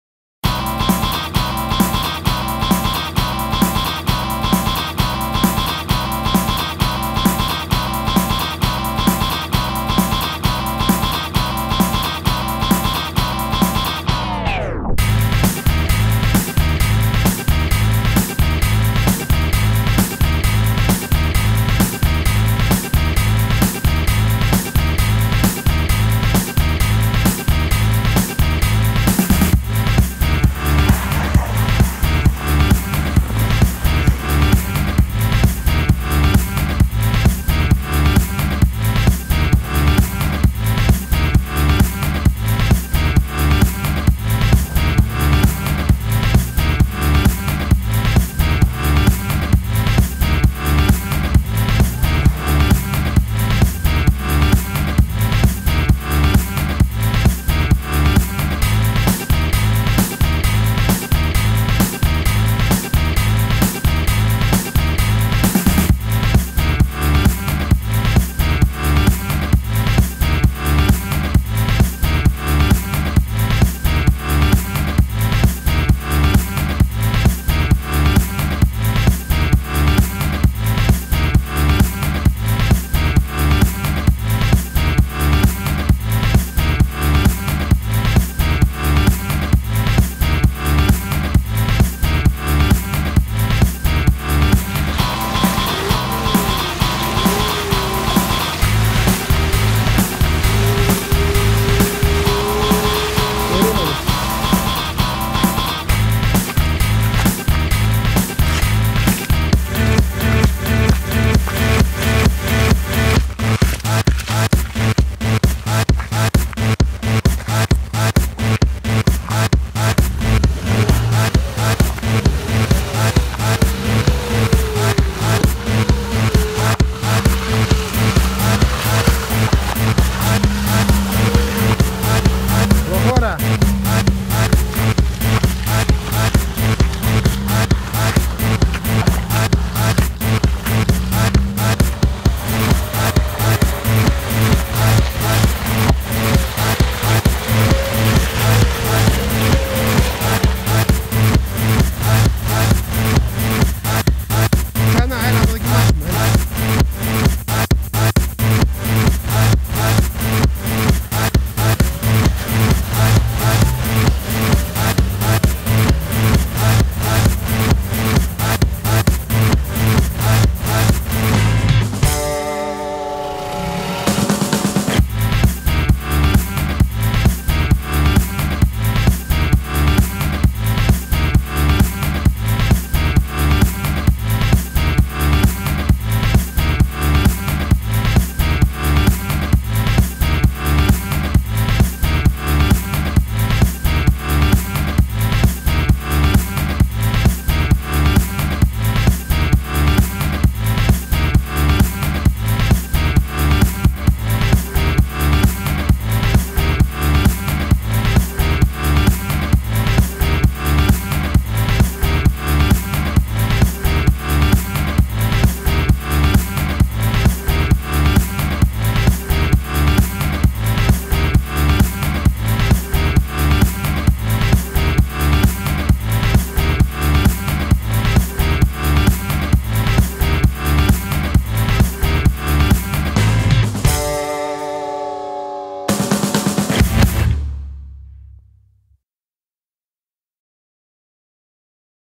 SUZUKI GRAND VITARA 2.0 2007 4X4 OFF ROAD snow TEST YOKOHAMA GEOLANDAR G015 - SORRY FOR THE MUSIC
NEW LINK WITH UPDATED MUSIC

Testing while playing in snow our Suzuki Grand Vitara with the new Yokohama Geolandar G015 tires.